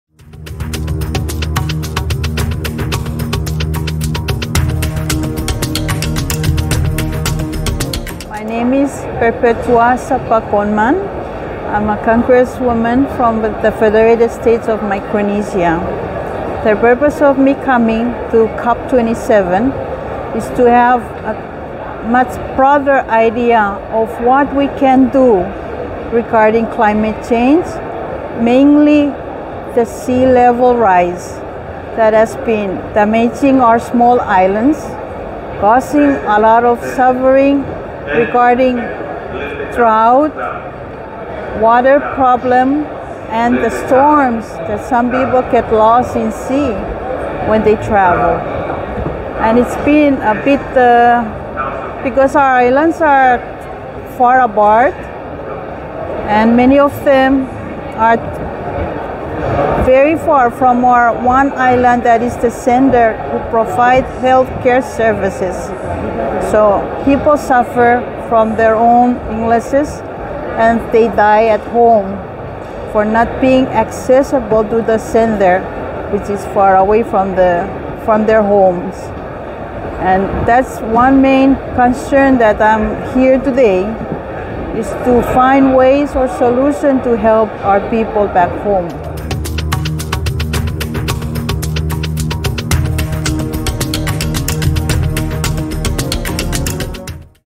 COP27: Perpetua Sappa Konman, Congresswoman (FSM)
As the 2022 United Nations Climate Change Conference ( COP27 ) proceeds, where countries come together to take action towards achieving the world's collective climate goals, we feature OurBluePacific leaders, activists, warriors, who represent and speak up for our islands.

Congresswoman Perpetua Sappa Konman from the Federated States of Micronesia, is in Sharm El Sheikh seeking solutions to relieve the plight of her people back home in FSM, as they face uncertain futures due to climatechange.